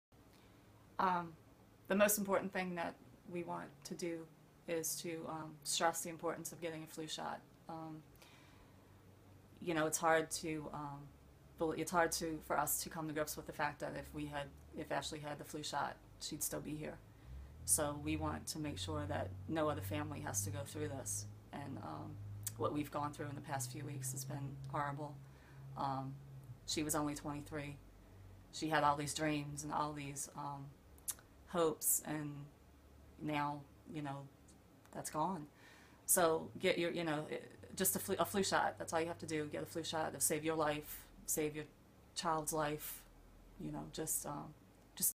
Patricia McCormick of Rochester spreads awareness about flu shots after her daughter, Ashley, died f
Patricia McCormick of Rochester spreads awareness about flu shots after her daughter, Ashley, died from the flu. @theoaklandpress